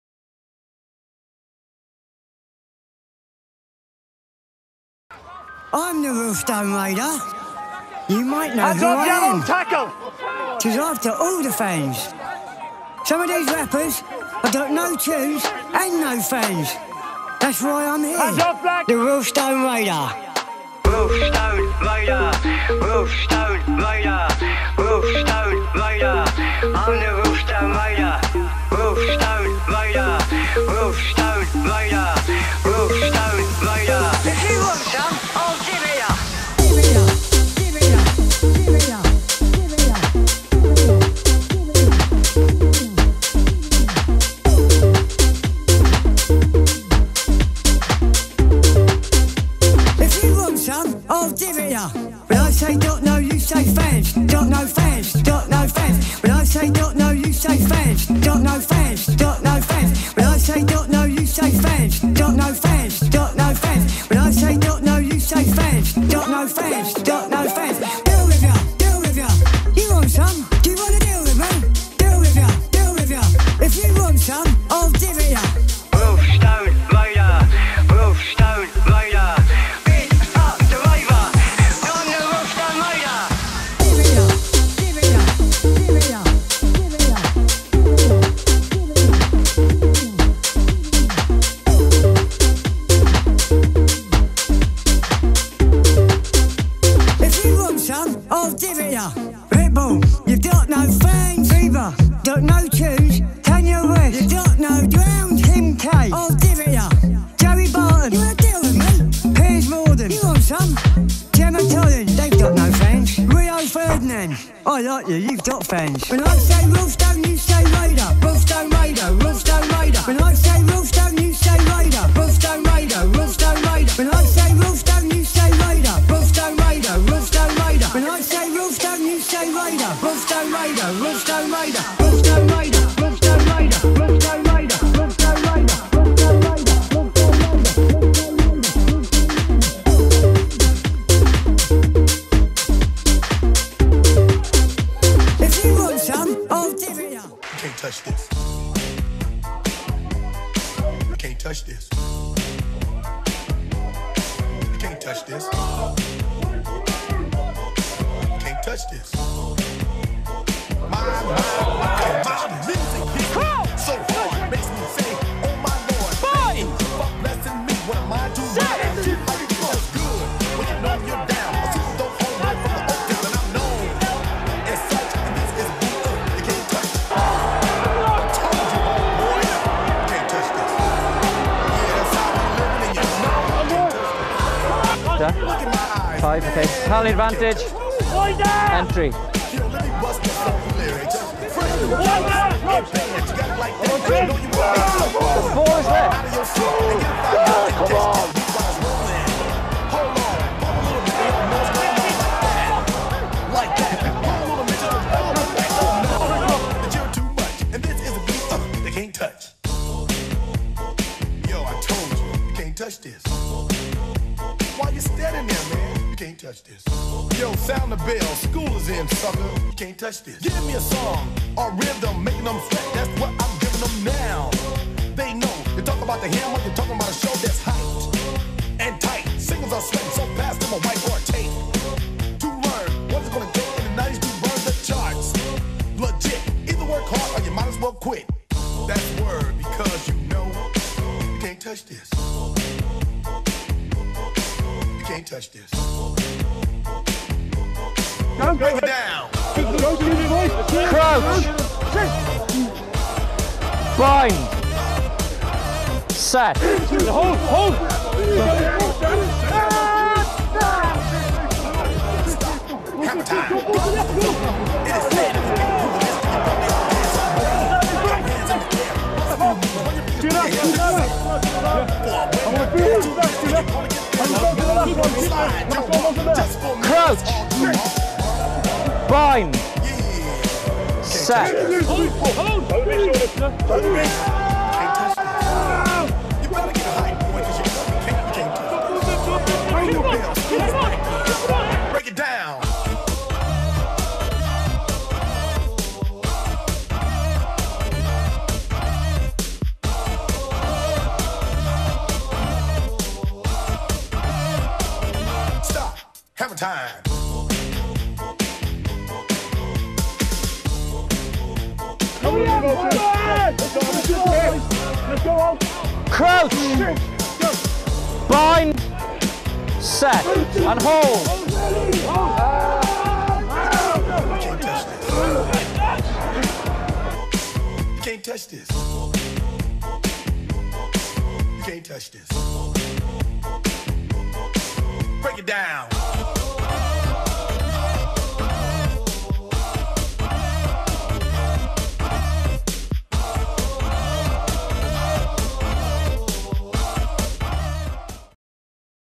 James Hall 16/17 Highlights
Thought Id share my 16/17 Season Highlights- Not had the most spectacular season and appearances have been limited. But I tried and enjoyed it when I got out there. Thanks to everyone at Bristol Rugby Club and those who have had to put up with me. Long road after my knee op.